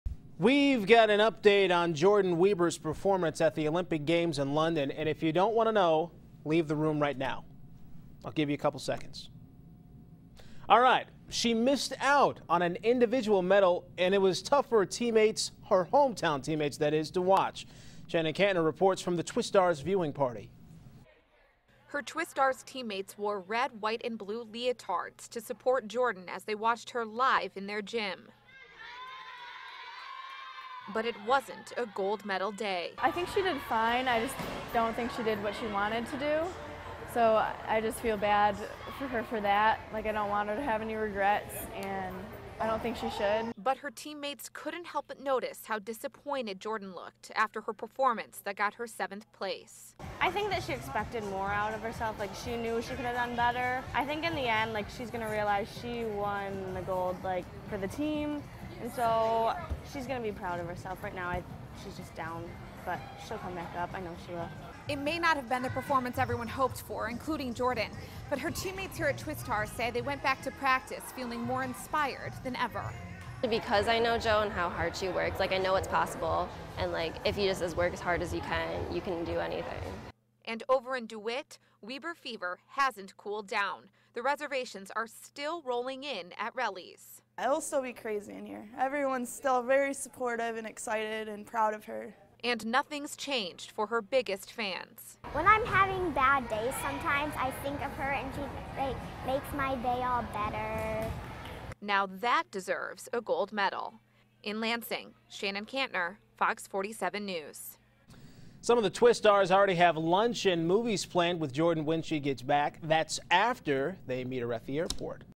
Teammates Watch Jordyn Wieber Miss Out on Gold
As hard as it is to hear Jordyn Wieber missed out on an individual medal Tuesday, it was even tougher for her hometown Twistars teammates to watch live in their gym.